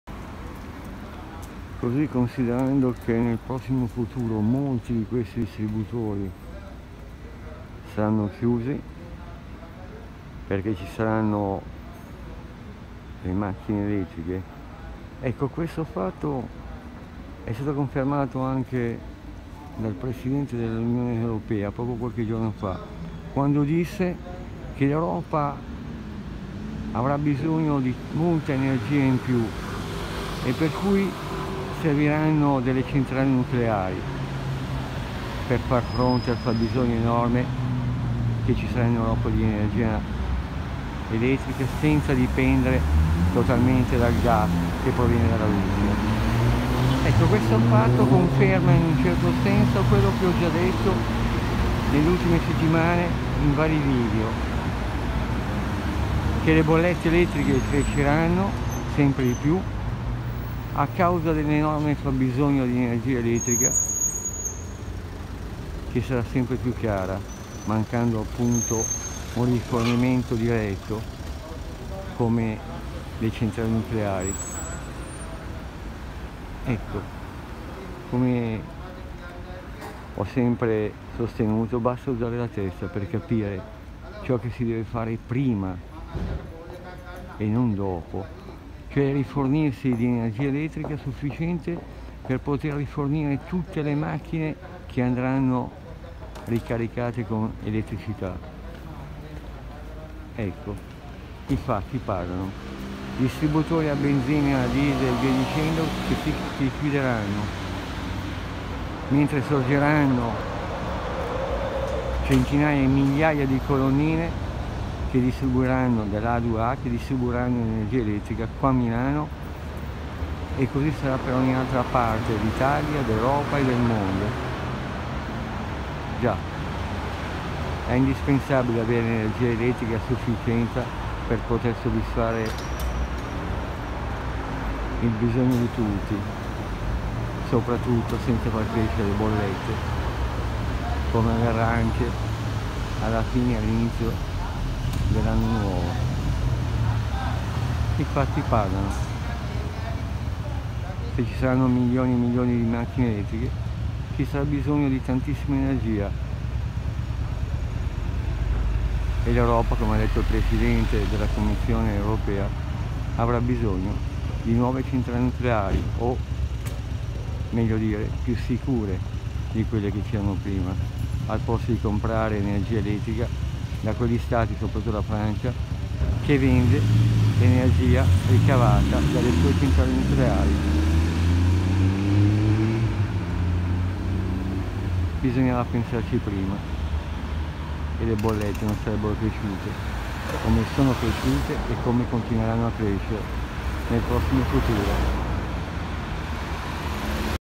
All’Europa servono Centrali Nucleari per avere più elettricità!
Settimana scorsa anche il Presidente della Commissione Europea Ursula Von Der Leyen ha detto che servono Centrali Nucleari per far fronte all’enorme bisogno di energia elettrica motivo per cui sono cresciute e cresceranno ancora le Bollette di Gas e luce!